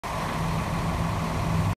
2010 PETERBILT 365 For Sale
2010 PETE 365 27' FLAT BED WITH MOFFIT SET UP CUMMINS 385 HP MOTOR AND 10 SP TRANS